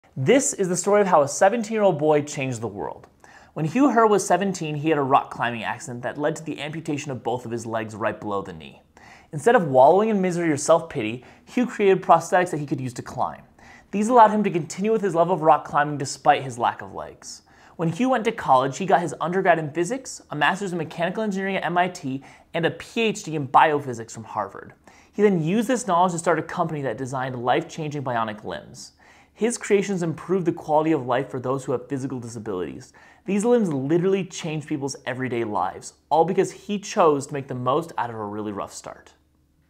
The 17 Year Old Who Changed The World
Hugh Herr turned tragedy into inspiration when he devoted his life to making better prosthetics after he lost both of his legs in a climbing accident. He has since changed the word of prosthetics and increased amputees' quality of life significantly. What challenges will you turn into inspiration?